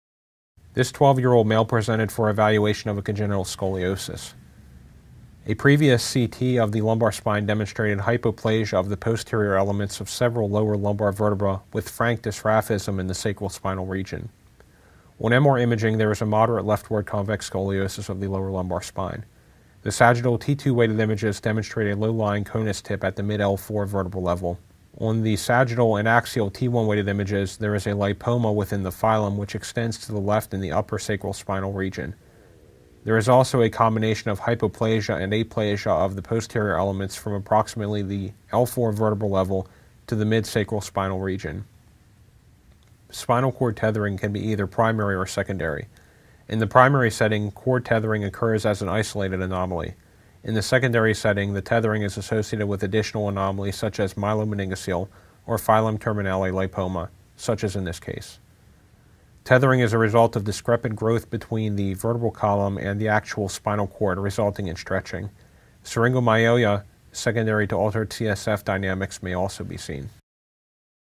Tethered Cord
This is a case of spinal cord tethering associated with a film lipoma. On MR imaging, there is a moderate leftward convex scoliosis of the lower lumbar spine. The conus tip lies at the mid-L4 vertebral level on the sagittal T2-weighted images. On the sagittal and axial T1-weighted images, there is a lipoma within the filum. There is also a combination of hypoplasia and aplasia of the posterior elements from approximately the L4 vertebral level to the mid-sacral spinal region. Spinal cord tethering can be either primary or secondary. In the primary setting, cord tethering occurs as an isolated anomaly. In the secondary setting, the tethering is associated with additional anomalies such as a myelomeningocele or film terminale lipoma, as in this case. Tethering is a result of discrepant growth between the vertebral column and the actual spinal cord resulting in stretching.